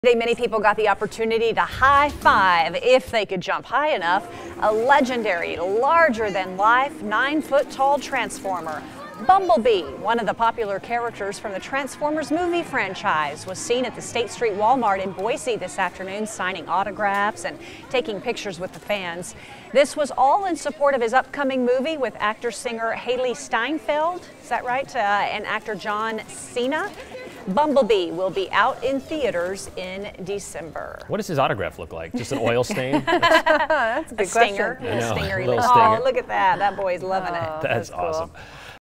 Bumblebee transformer visits Boise
One of the popular characters was spotted at a Walmart store on State Street.